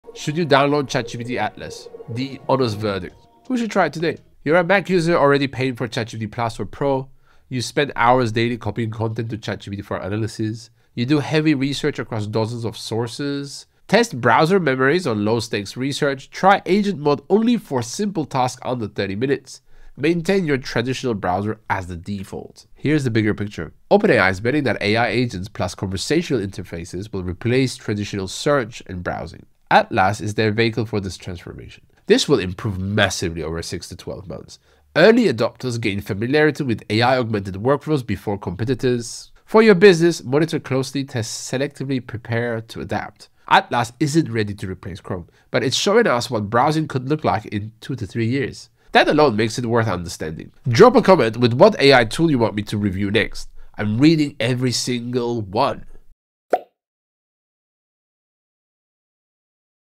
Should You Download ChatGPT Atlas?
OpenAI launched ChatGPT Atlas, and it might be the most ambitious browser experiment we’ve seen—agent mode, browser memory, and a new way to work with AI.
But should you actually download it?
In this video, I break down:
What agent mode really does (and when it fails)
Why browser memory could change how we do research
Major security risks no one is talking about
My honest verdict on when to use it—and when to skip it
If you’re a Mac user already deep in the ChatGPT ecosystem, Atlas might give you an edge. But for most teams and businesses, the real benefit comes from knowing where browsing is headed.
🔍 AI won’t replace Chrome just yet—but it’s pointing to the future of work.
📩 Drop a comment with the next AI tool you want me to review.